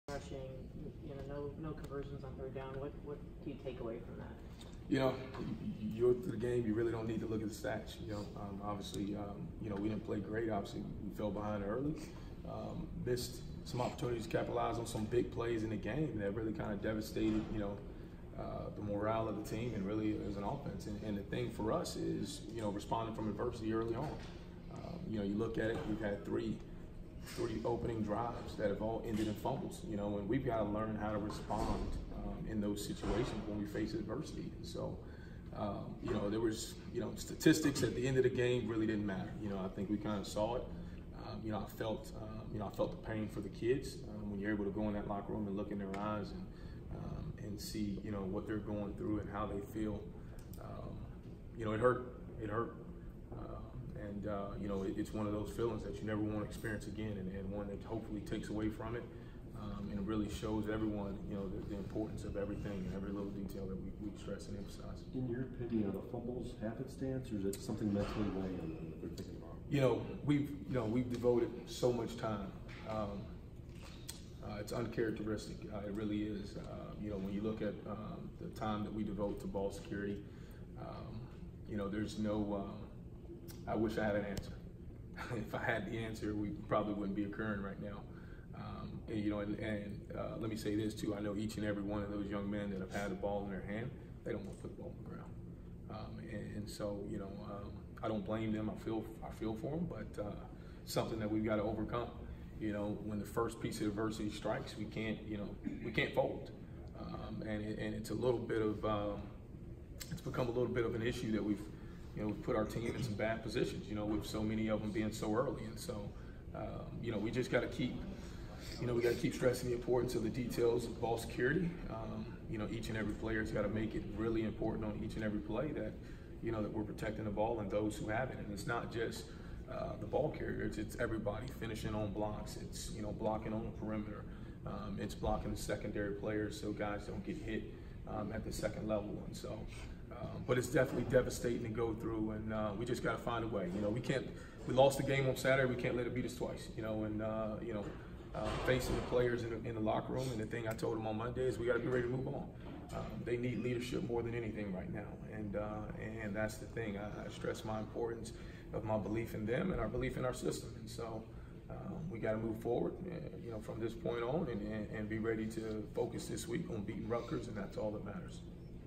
Josh Gattis: Michigan can't fold at first hint of adversity
Michigan football offensive coordinator Josh Gattis speaks to the media on Wednesday, Sept. 25, 2019, in Ann Arbor.